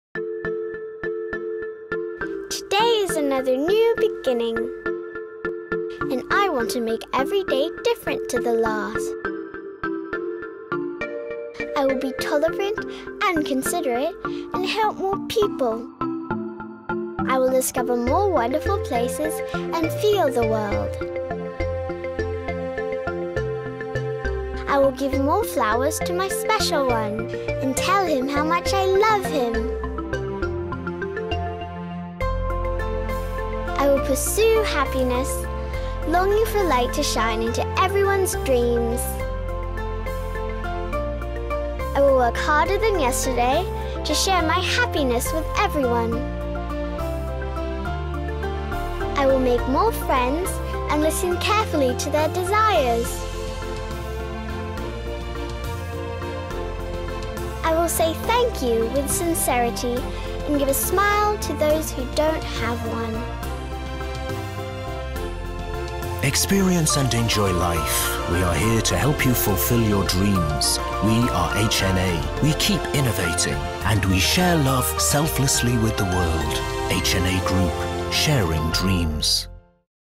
HNA Group China
A brand point of view film for the HUGE HNA Group, 170 in the Fortune Global 500 list worth $53bn. Concept by Chris Catchpole at Interone China.